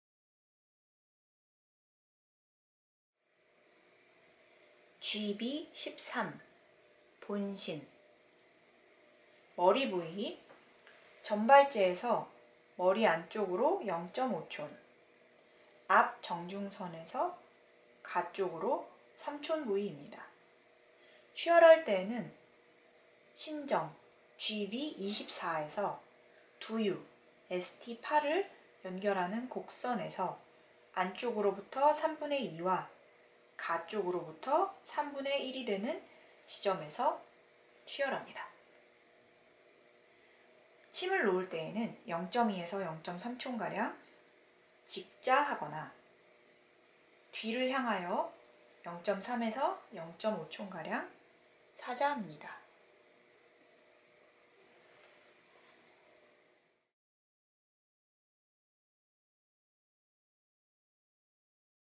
[KMCRIC 표준경혈 DB] GB13 본신(本神)
[부위]
머리, 前髮際(anterior hairline)에서 머리 안쪽으로 0.5寸, 앞정중선(anterior median line)에서 가쪽으로 3寸.

On the head, 0.5 B-cun superior to the anterior hairline, 3 B-cun lateral to the anterior median line.

[취혈]
神庭(GV24)에서 頭維(ST8)를 연결하는 곡선에서 안쪽으로부터 2/3와 가쪽으로부터 1/3이 되는 지점에서 취혈한다.

Note: GB13 is at the junction of the medial two thirds and lateral one third of the curved line from GV24 to ST8.

[침구법]
- 直刺 0.2～0.3寸
- 斜刺 0.3～0.5寸 (뒤를 향하여 沿皮刺)

★ 한의학 뉴스, 학회, 연구 동향 등 더 풍부하고 다양한 한의학 정보를 한의약융합연구정보센터 홈페이지에서 만나보세요.